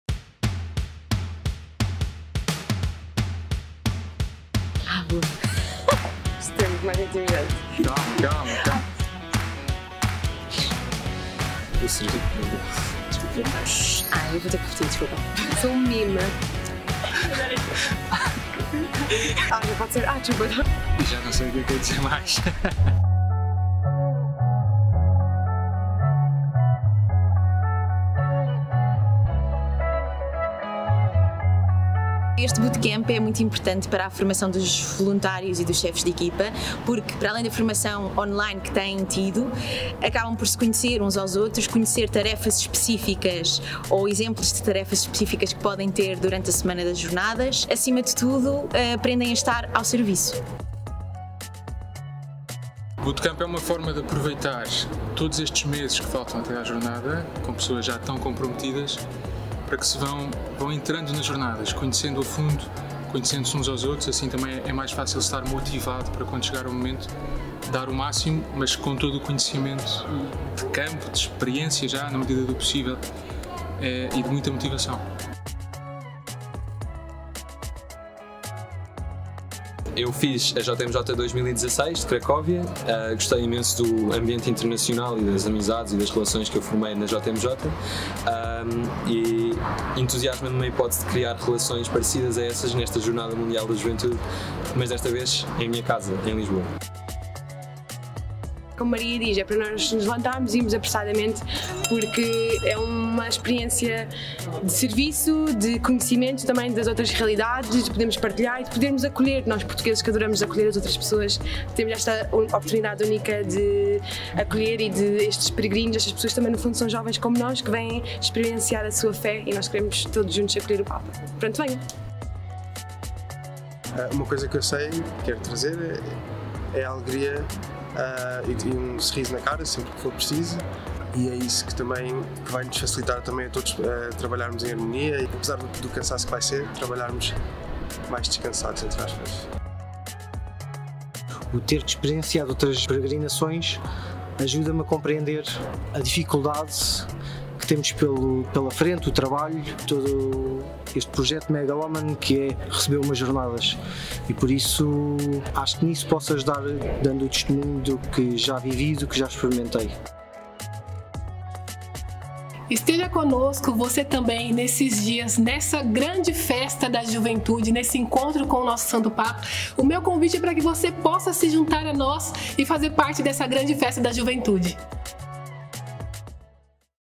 Bootcamp - L'expérience d'être volontaire
La deuxième édition du WYD Boot Camp a eu lieu à Lisbonne, où les volontaires et les chefs d'équipe ont reçu une formation essentielle afin de pouvoir remplir au mieux leurs fonctions.
Les outils fournis lors de ces bootcamps permettent "aux volontaires d'apprendre à connaître l'événement et l'organisation, la tâche qui leur est confiée, la manière dont ils doivent la réaliser, et de faire connaissance avec les autres volontaires avec lesquels ils font équipe".